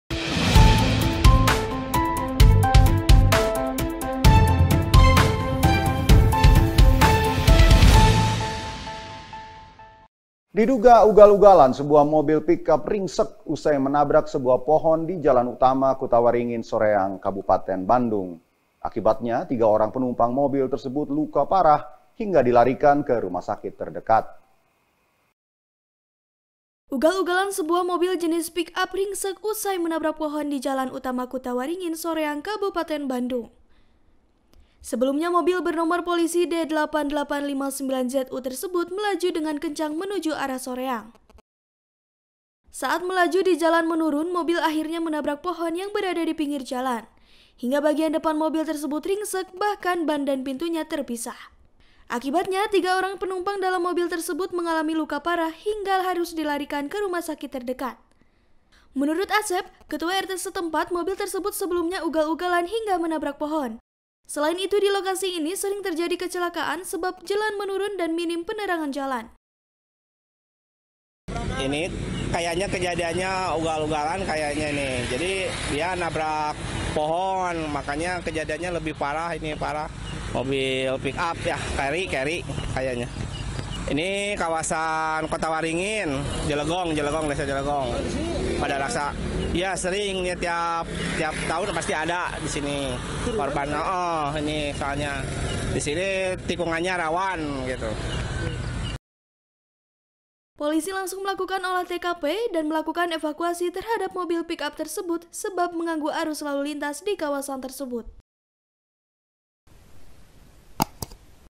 Ugal-Ugalan Mobil Pickup Ringsek Usai Menabrak Pohon | UN News | UTV
Diduga ugal-ugalan sebuah mobil pickup ringsek usai menabrak sebuah pohon di jalan utama kutawaringin - SOreang Kabupaten Bandung. Akibatnya tiga orang penumpang mobil tersebut luka parah hingga di larikan ke rumah sakit terdekat.